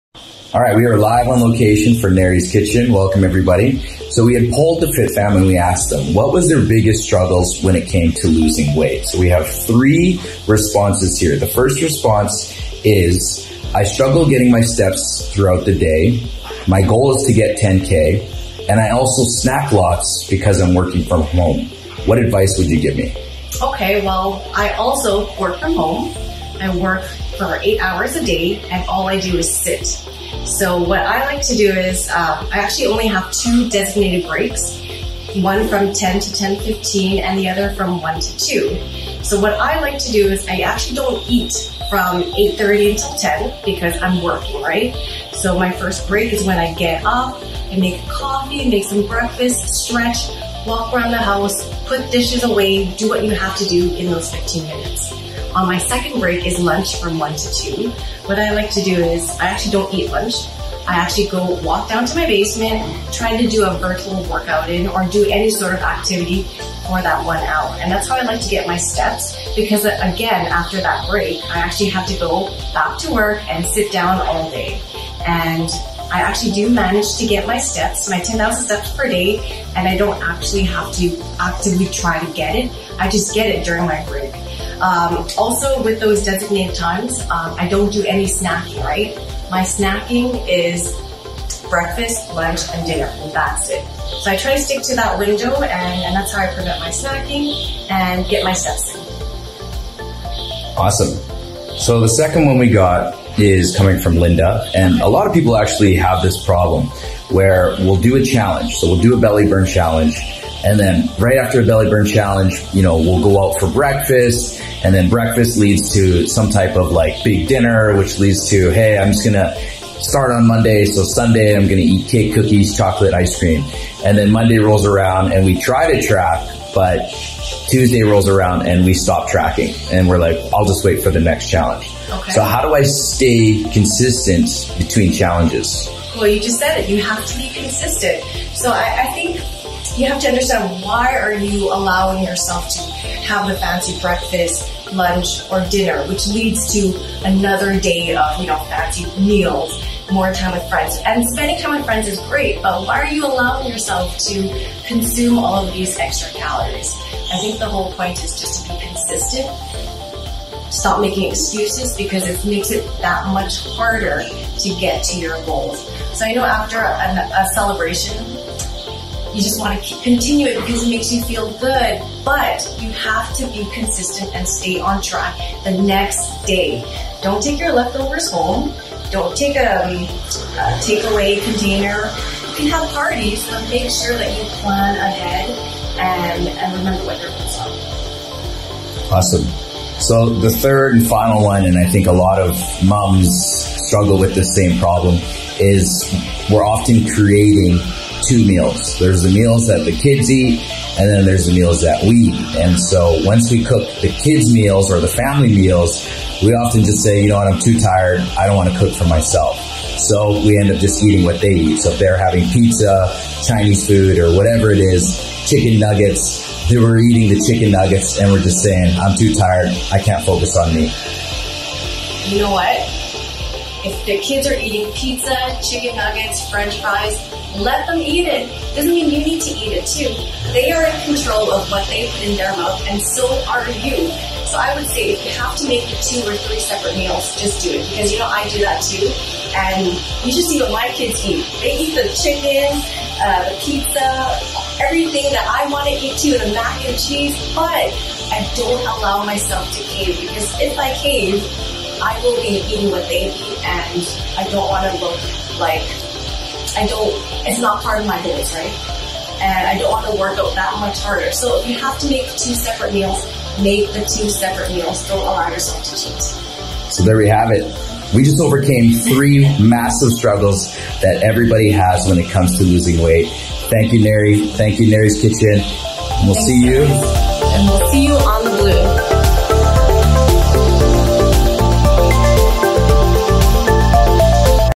Biggest Struggles To Losing Weight
Do you work from home, don't move and find yourself snacking?

Are you a frequent challenger but find yourself falling off between challenges?

Are you forced to make separate meals for the family and yourself, but then don't eat what you should eat and eat what eat?

These are all legitimate problems that do get in our way of hitting our dream goal.

Today in Nary's kitchen we give the solution to the biggest struggles when it comes to losing weight.